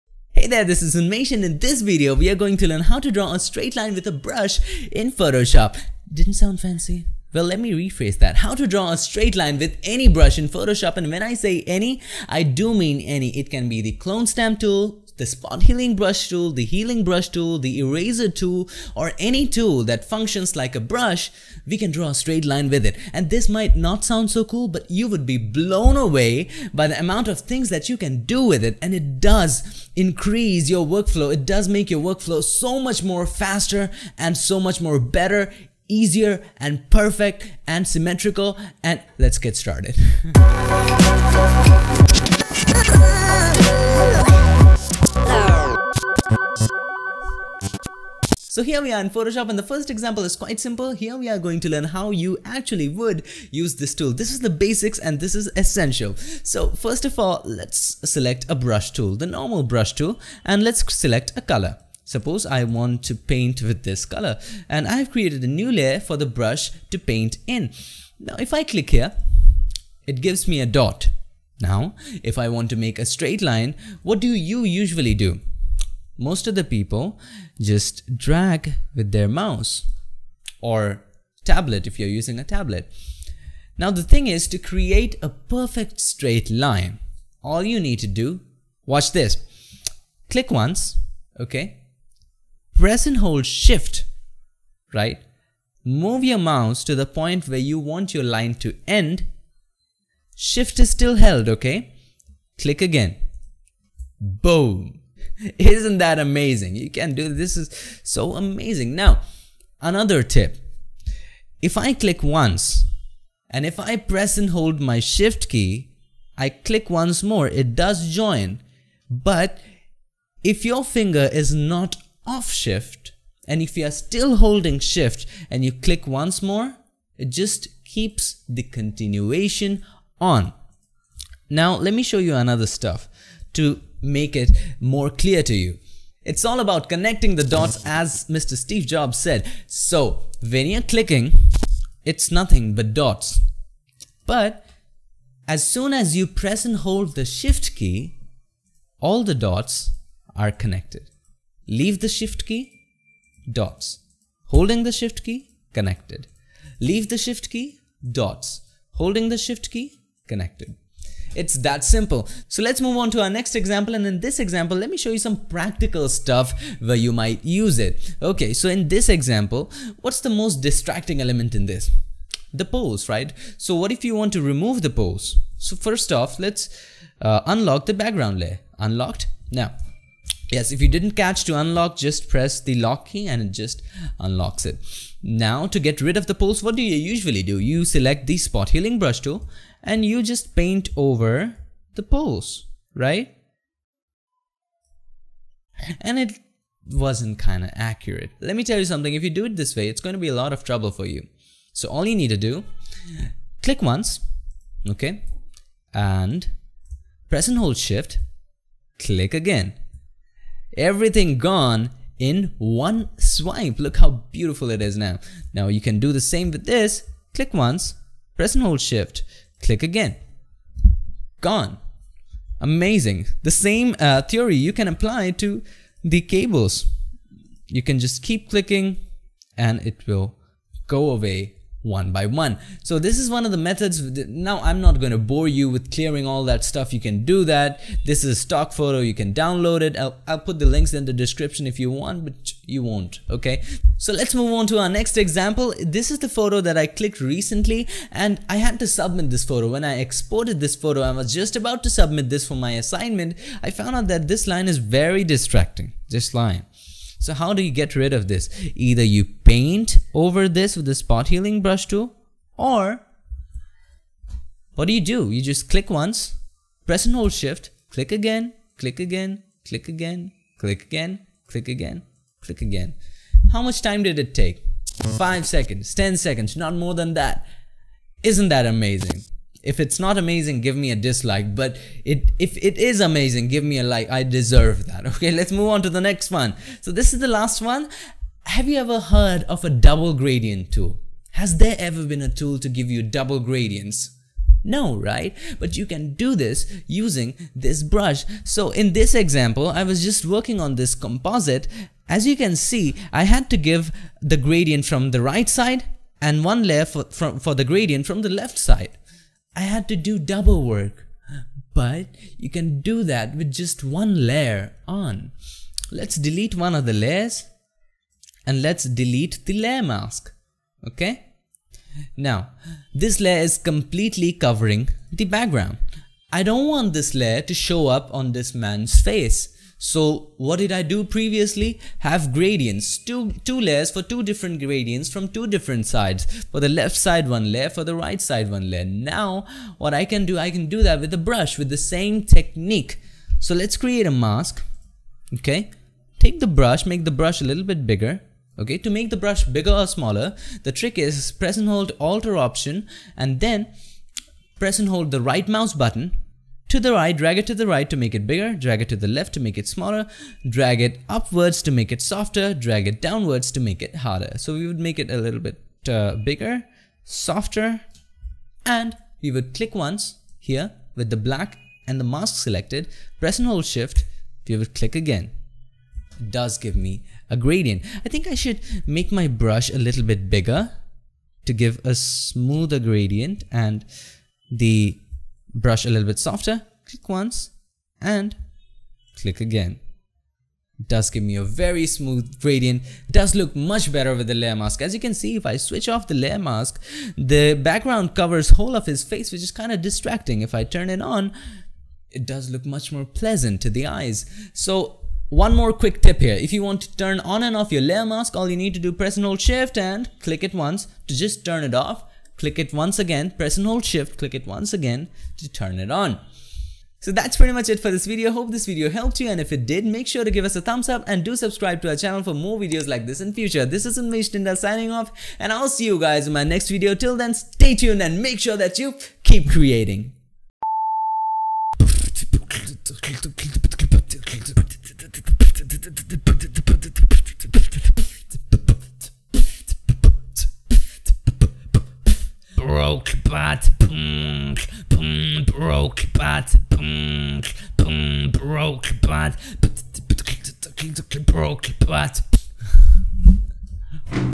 Draw Straight Line with Any Brush in Photoshop
Learn how to paint in straight lines with ANY BRUSH using this amazing shortcut. Either it is the Clone Stamp Tool, the Spot Healing Brush Tool, the Healing Brush tool or even the eraser, you can paint in straight line with every tool that functions like a brush using this very useful shortcut.

A straight line in Photoshop will be very useful and precise not only in using a normal brush for a piece of art. There's much more to it. Suppose you want to remove cable lines from image with the spot healing brush tool. Dragging the brush manually can be inaccurate. Using this technique you can remove all of those cables and towers using just a few clicks as shown in this video.

Though there are lots of uses of this shortcut, which you will discover soon, one of them, which I have discussed in the video, is double gradients.  You can use this technique to get gradients on both sides instead of just in one direction.

Making a straight line with a brush is very simple. All you need to is, select the brush and determine the size, color (if applicable), flow and opacity. Then, click with the brush at the point where you want your straight line to begin, then, press and hold shift. With Shift held down, click with the brush at the point were you want your line to end and, you will have a straight line. Now, if you want your brush to take a turn and bend, keep holding shift and keep clicking in the direction you desire.